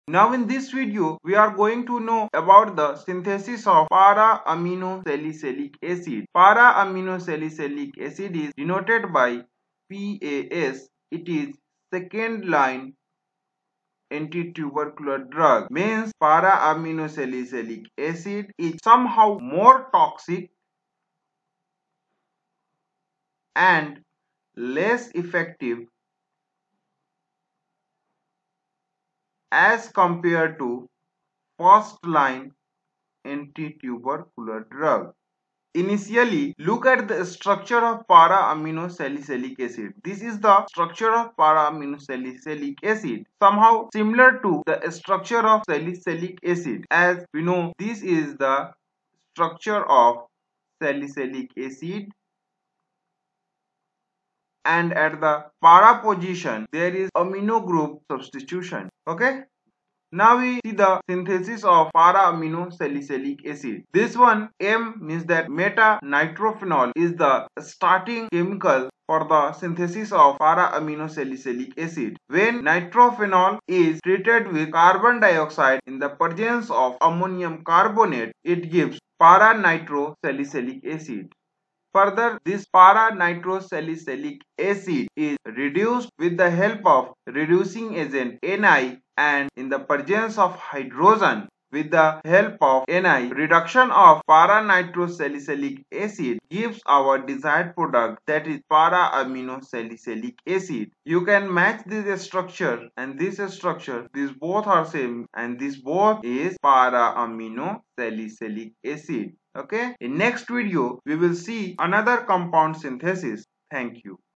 Para-Amino Salicylic Acid Synthesis | PAS| English | Pharmacy | Easy
In this video, Synthesis of P-amino Salicylic acid is taught.
PAs is 2nd line anti-tubercular drug used in the treatment of Tuberculosis caused by Mycobacterium Tuberculosis (rich in mycolic acid).
This drug is used mainly when there is resistance occurred by using 1st line anti-tubercular drug.

Keep Supporting.